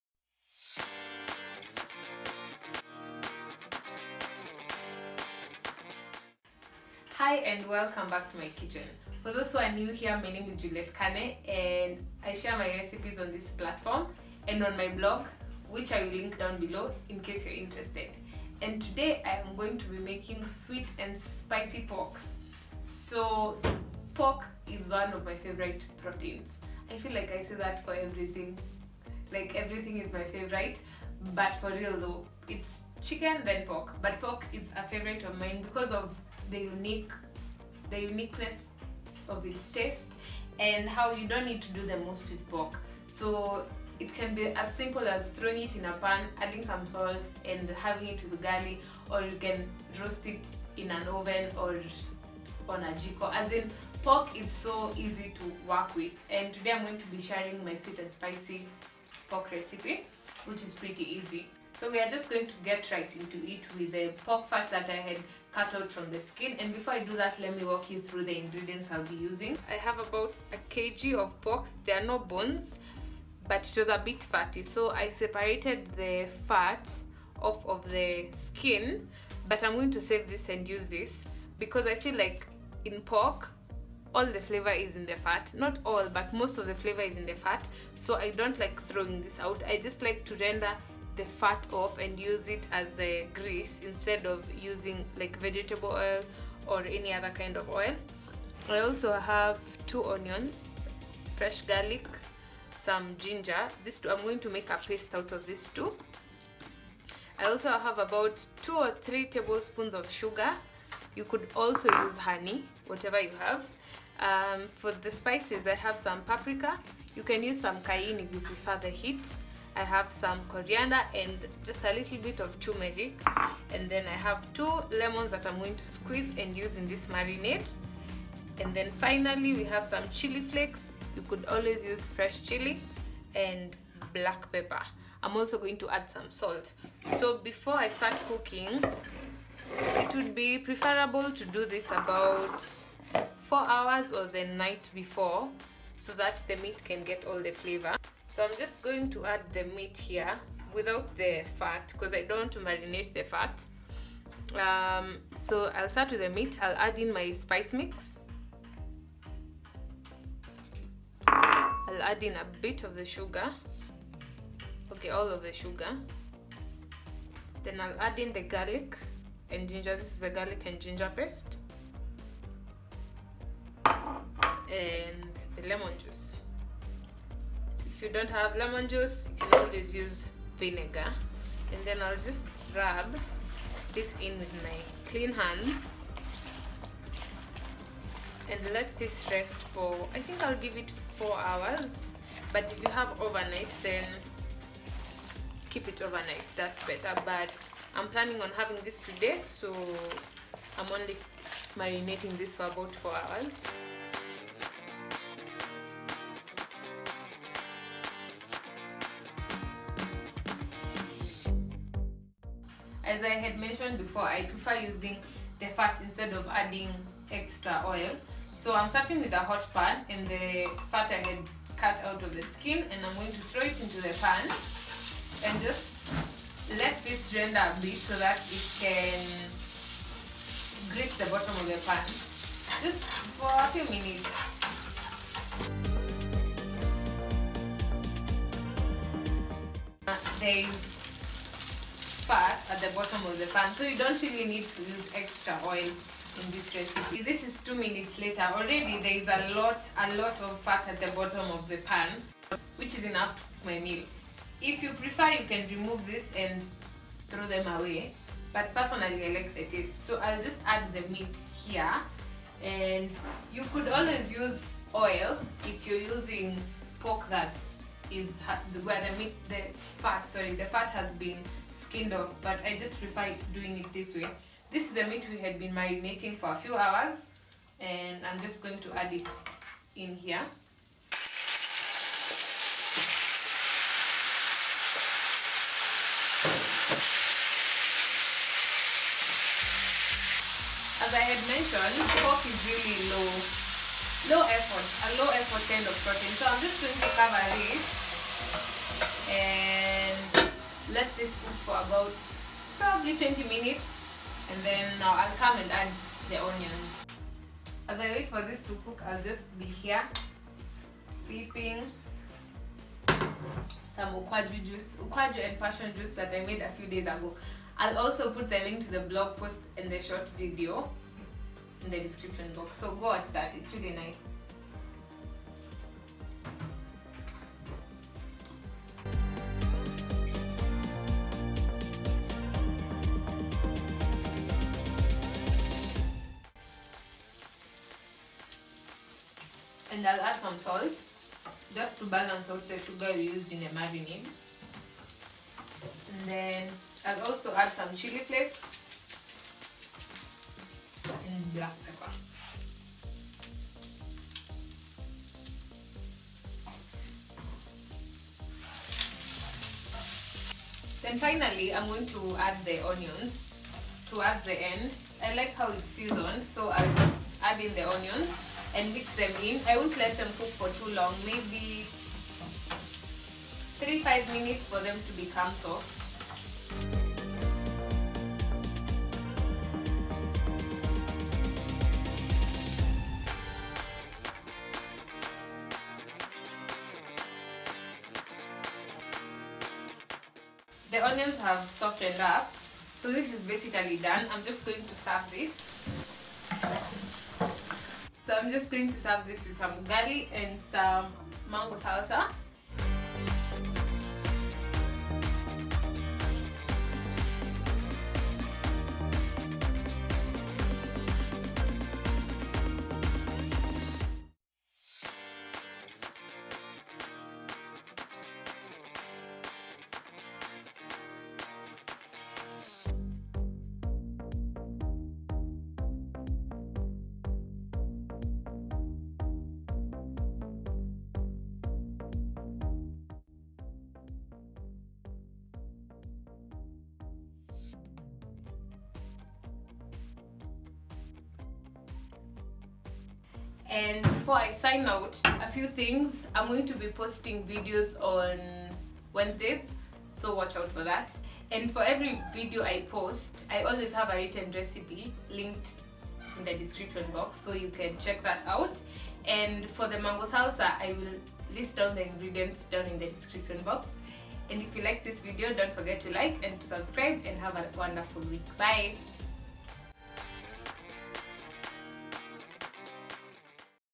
Sweet and spicy pork |Kanes Kitchen Affair
This turned out to be super delicious!!

Ingredients for the salsa
1 mango diced
2 small tomatoes
1 finely chopped onion
1 freshly squeezed lemon
Dhania/coriander
Pinch of salt
Blackpepper.

Filming: Canon 600D with a 55mm f1/4 USM Lens and iphone 7.

Bye:))